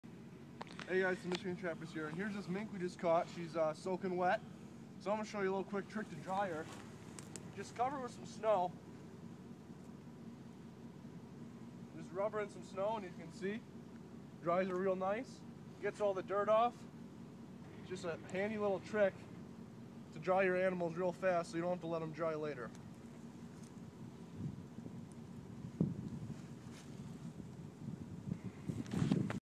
Trappers Trick: How to Dry Wet Fur
Just a little trick on how to dry fur fast! Works great and is very efficient!